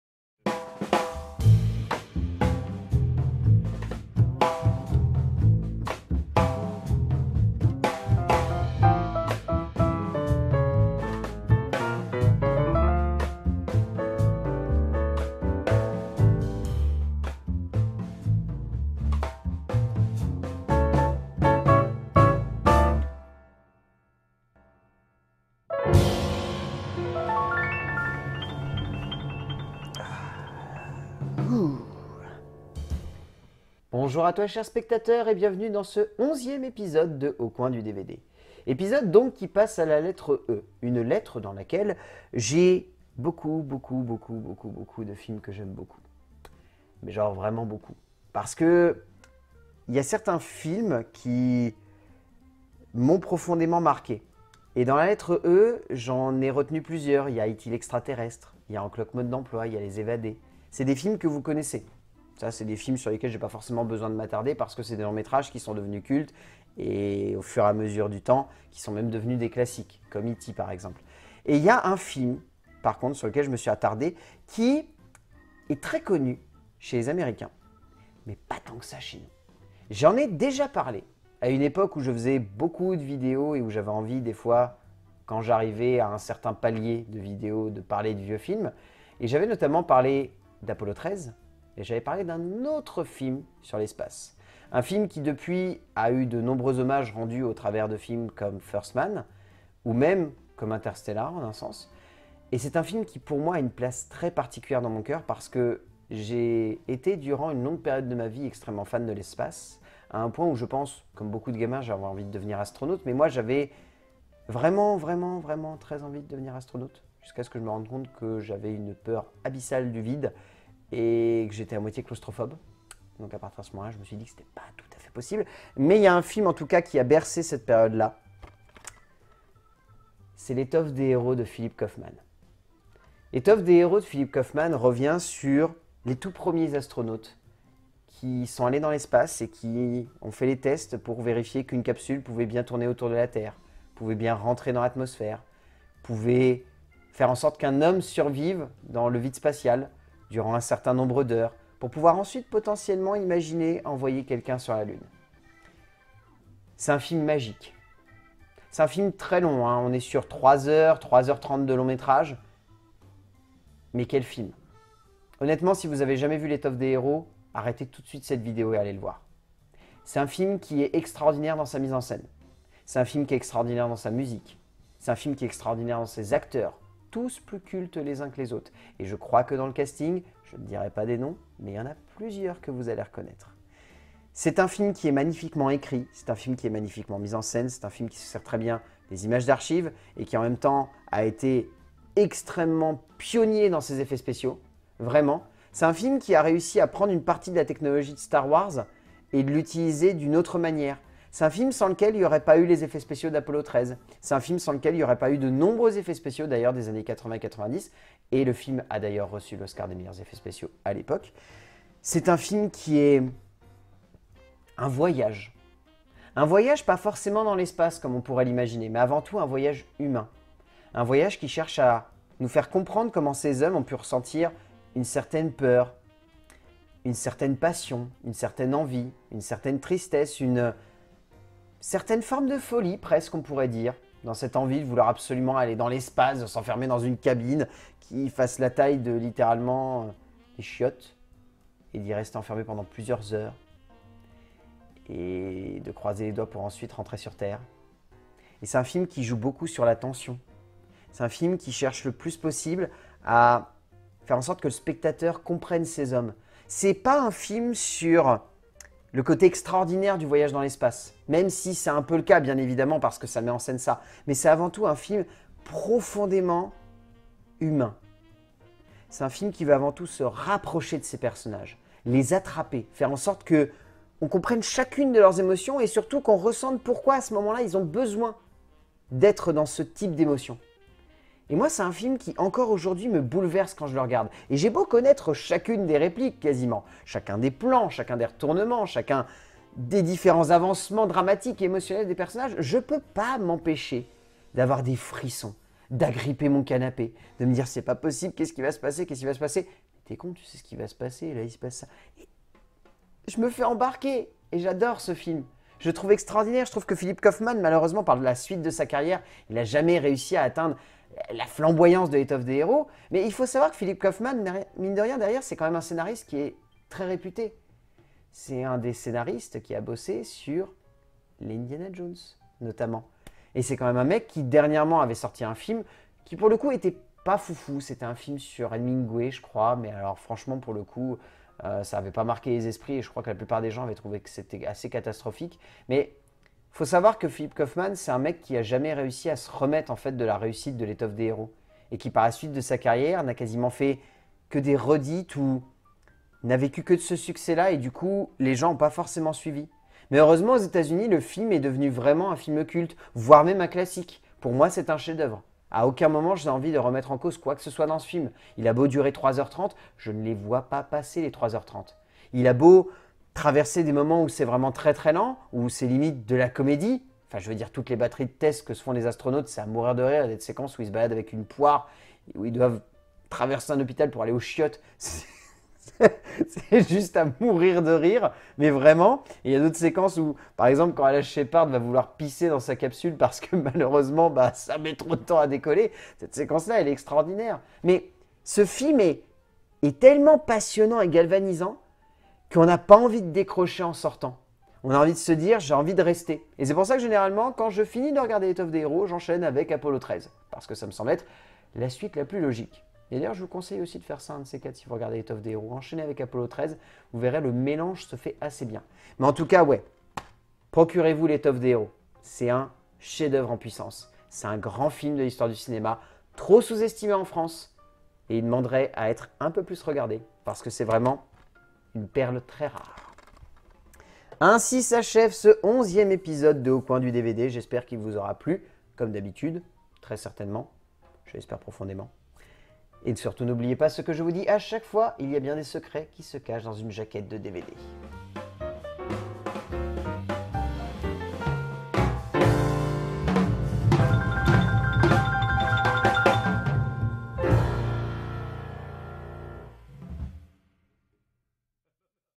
Au coin du DVD Ep.11 - L'étoffe des héros
Dans ce nouveau concept, nous chercherons à explorer ma DVDthèque, afin que vous découvriez quelques petites pépites que je garde caché dans celle ci
Cette semaine, je vous invite à partager avec moi, un voyage rare, un voyage unique, celui des premiers hommes, à avoir tenter de conquérir l'espace, "L'étoffe des héros"